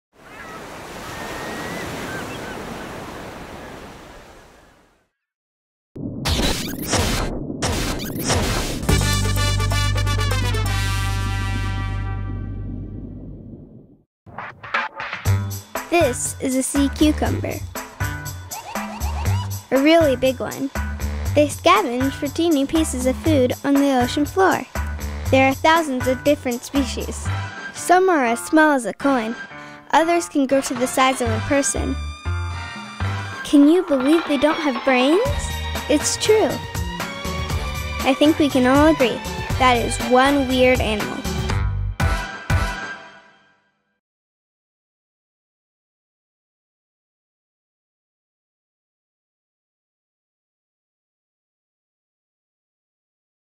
Weird Animals: Sea Cucumber
This is a Sea Cucumber -  a really big one!

They scavenge for tiny pieces of food on the ocean floor.

There are thousands of different species.

Some are as small as a coin, others can grow to the size of a person.

Can you believe they don't have brains? It's true!

I think we can all agree - that is one weird animal!

Links/Credits:
National Marine Fisheries Service: Weirdfins: